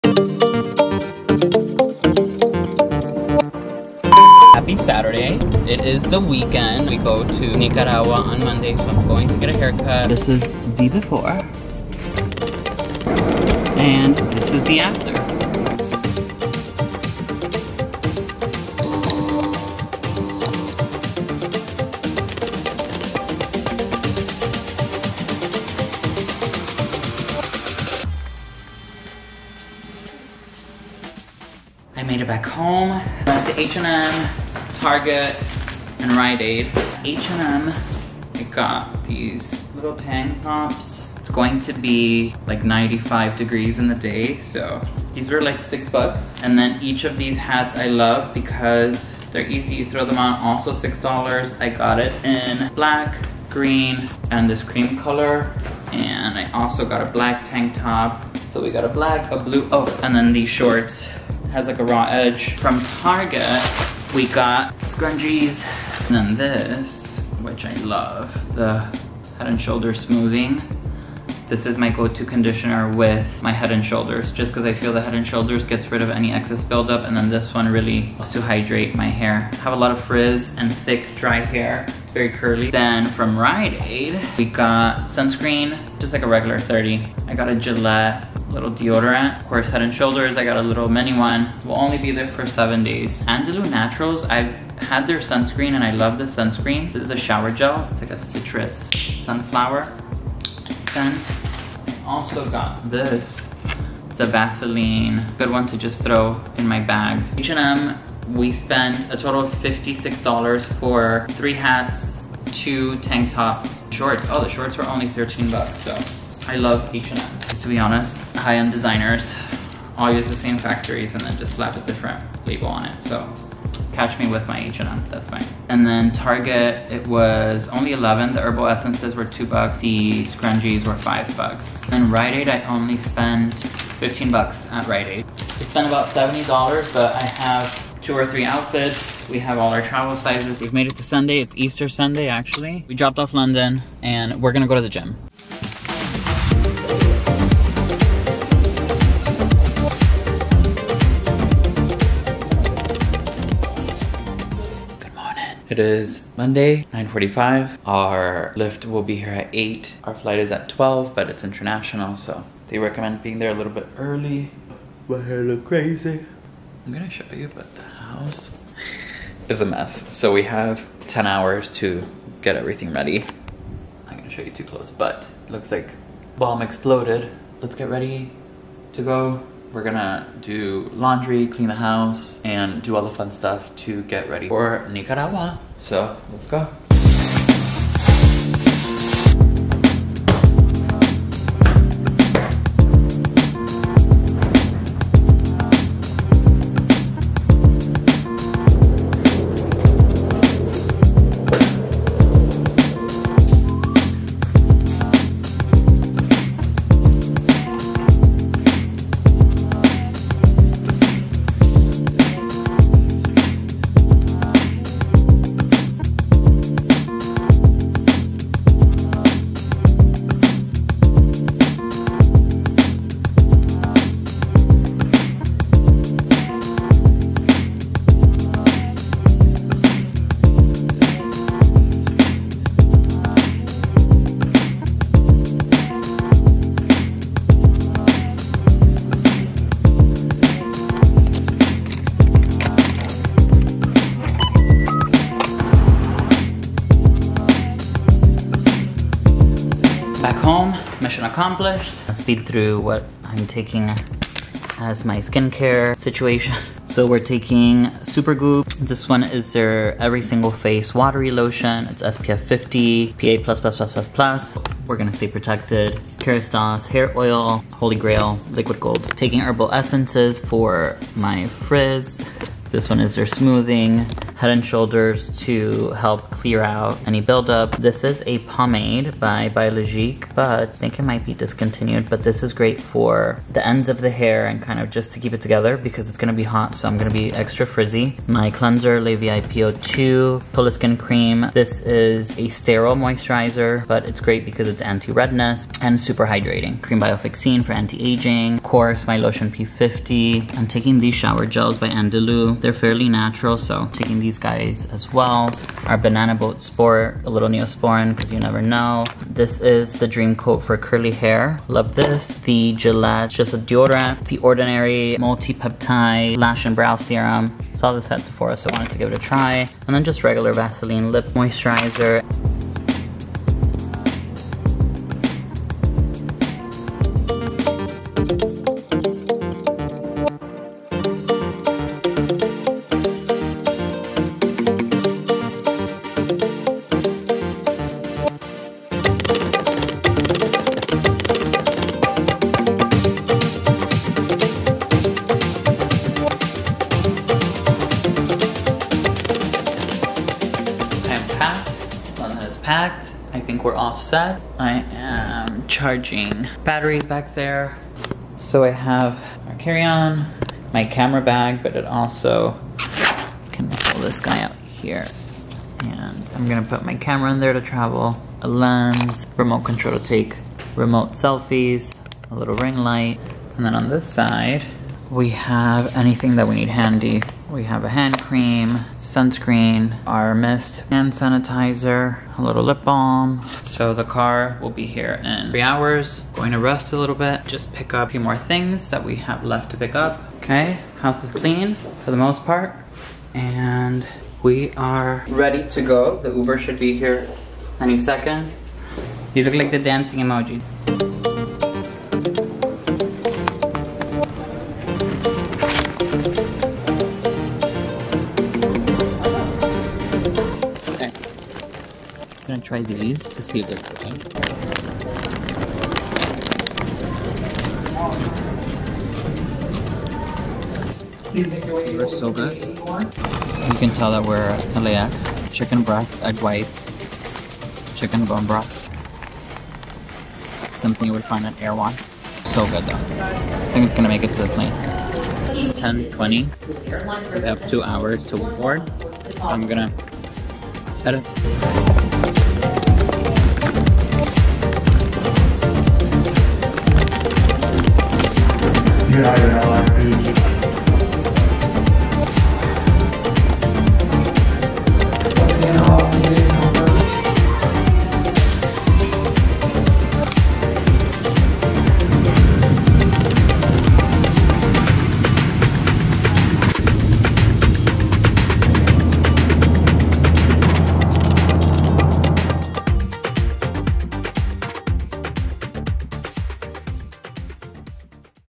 Get Ready For Nicaragua With Me! Packing Skincare And Travel Essentials.
Get ready with me to go to Nicaragua for 10 days! I share packing for vacation and skincare travel essentials. Excited for you to see the Nicaragua vlogs! xx

Instagram and TikTok @richhvision